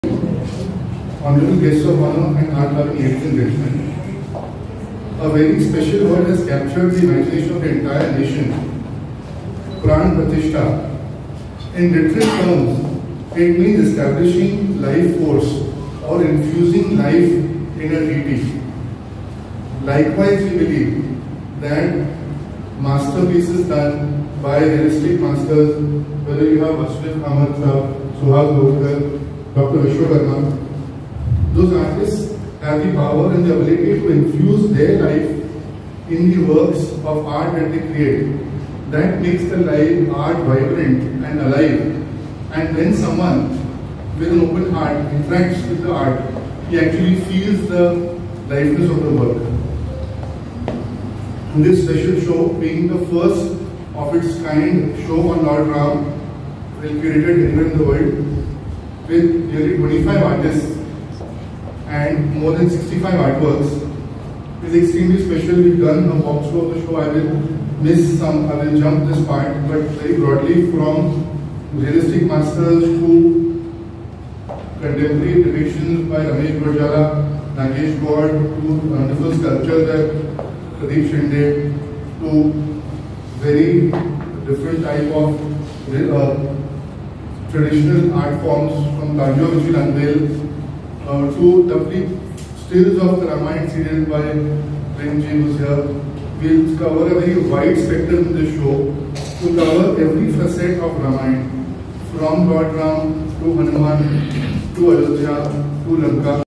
Aura Art Ram Show - Opening Comments by Rishiraj Sethi
Sharing the Opening comments by Rishiraj Sethi at the inauguration of the ArtShow MaryadaPurushottamRAM, going on at AuraArt Gallery till January 28, 2024.